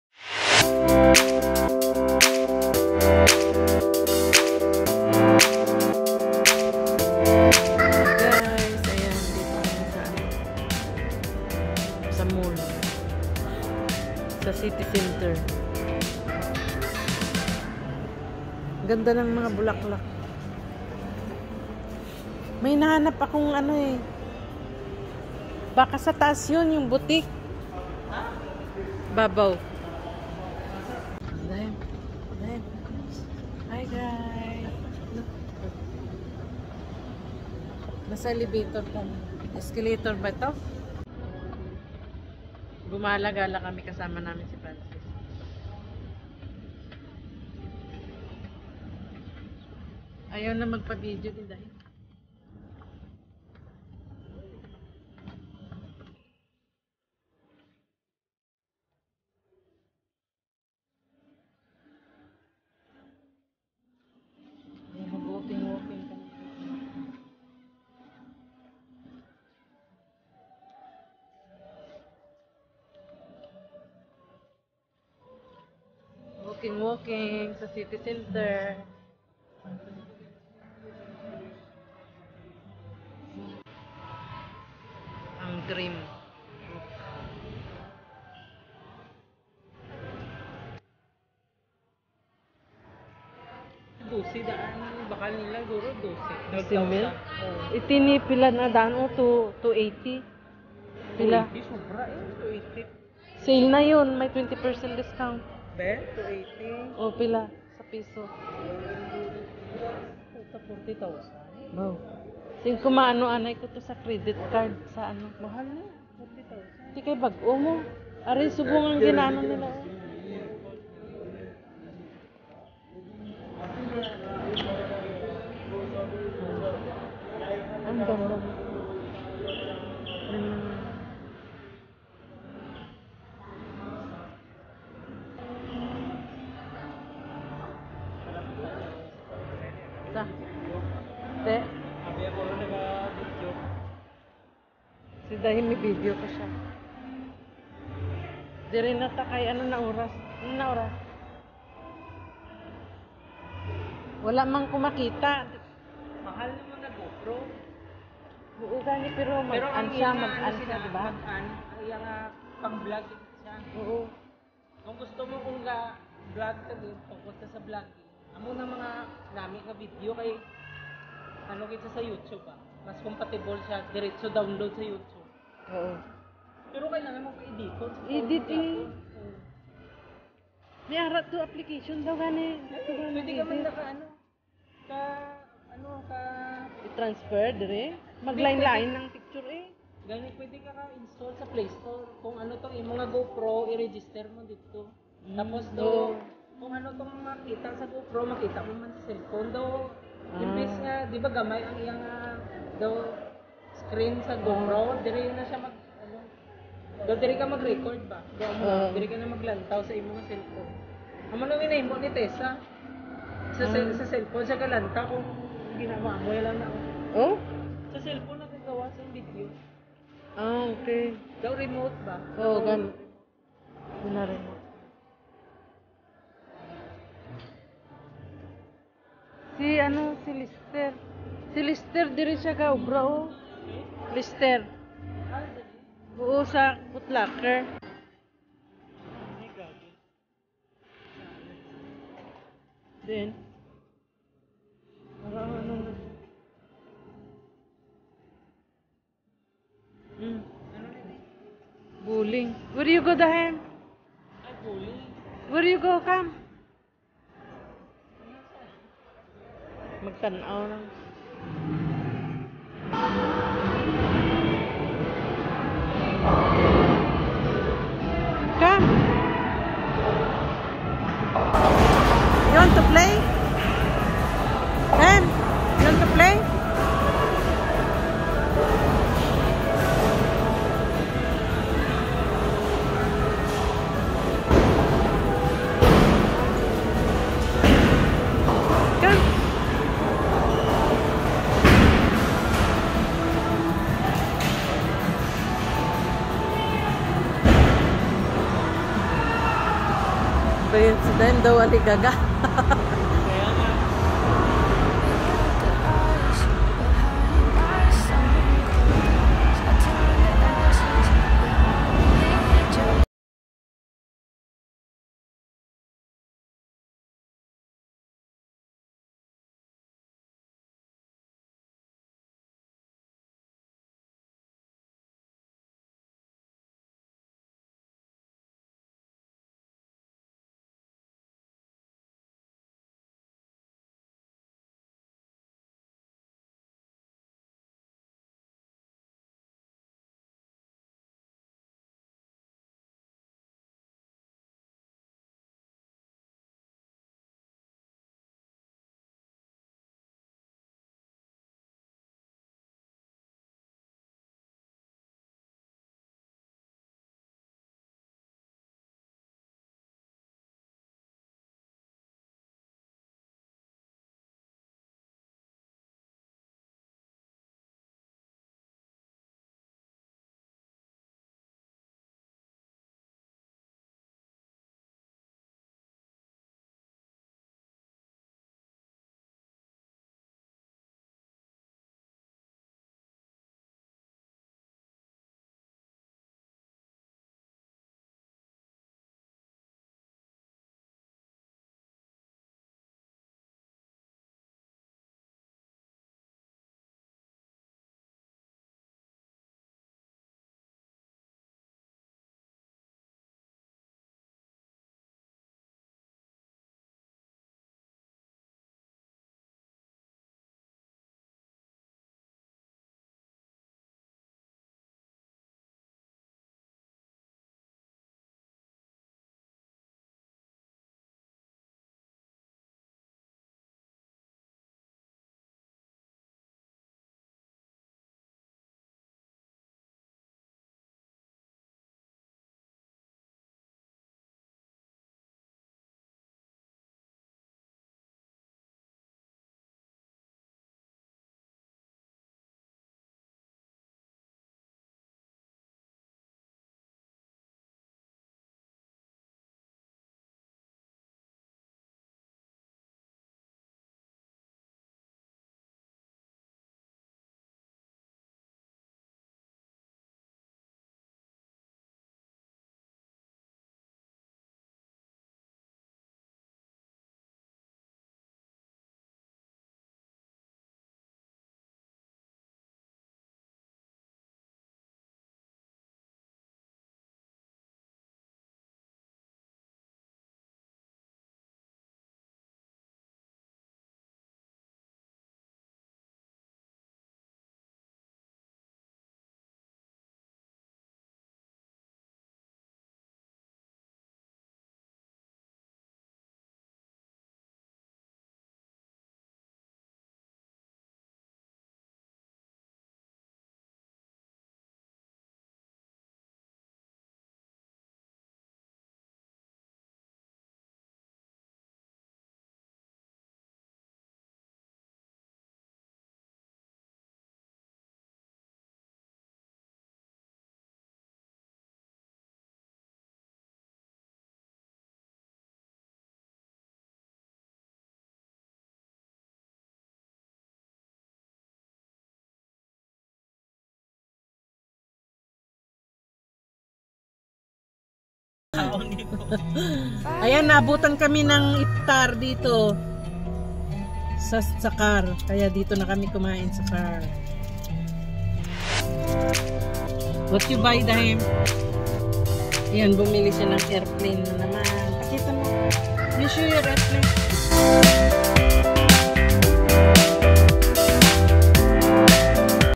visiting the bahrain city center during Ramadan time
gumala gala kmi during Ramadan time together with dahim and Francis

Musician: Shades